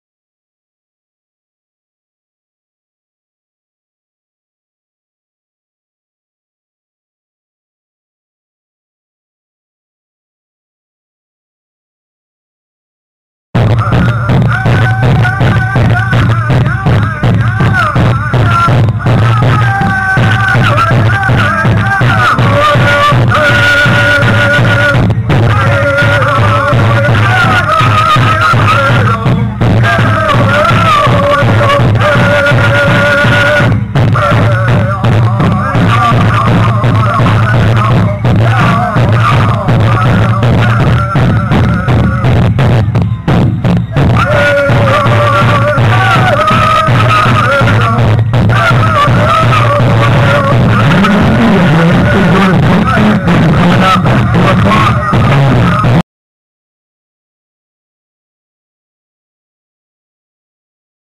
Quiet Bear Straight Dances at Auburndale
Quiet Bear doing Southern Straight Dance at the 2008 Auburndale Pow Wow